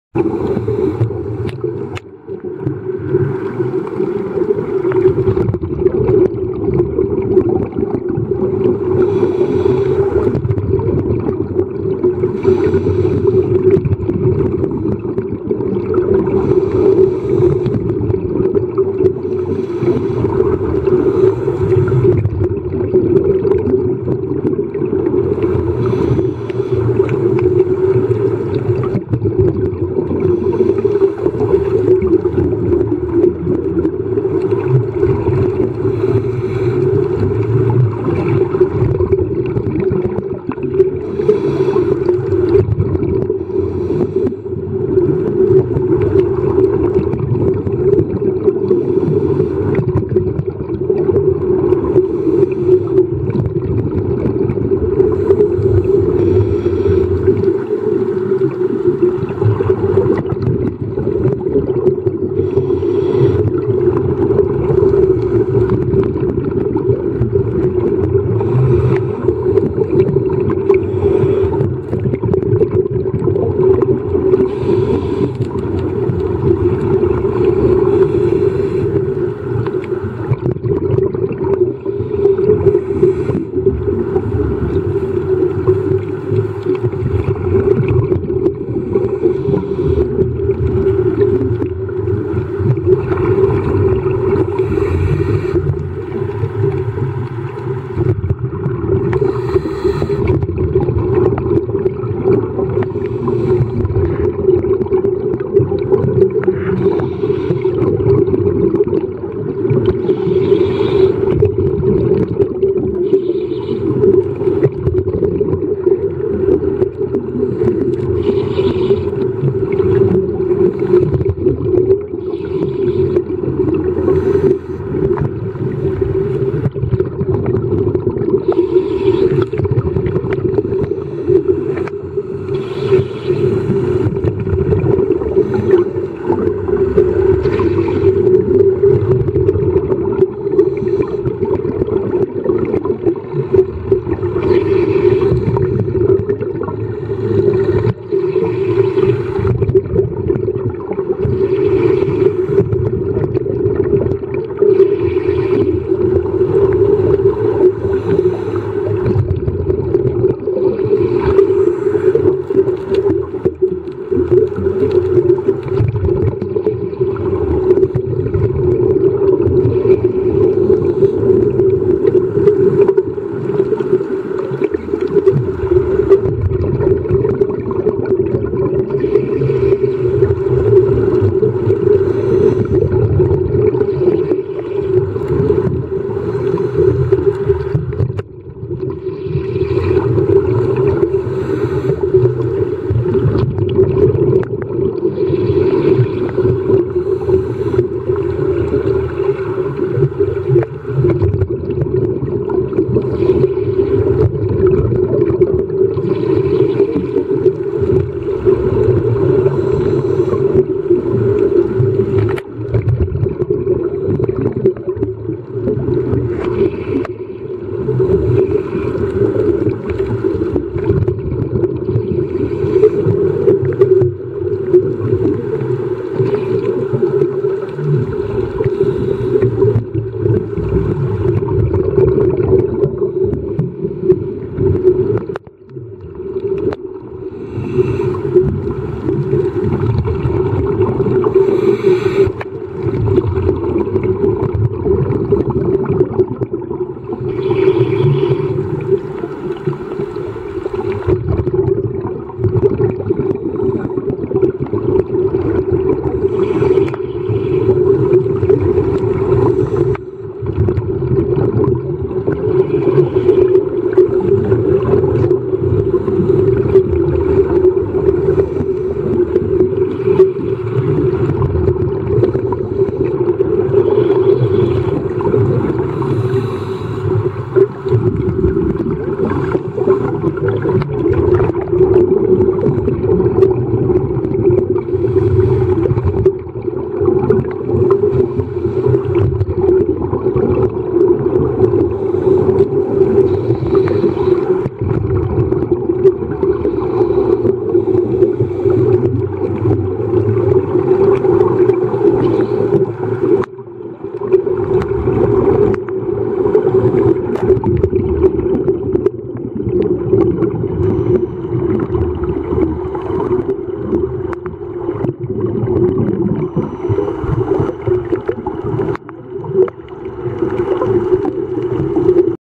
Feeding a sea turtle in Palawan, Philippines
Feeding a beautiful sea turtle sea grass at Club Paradise, Palawan.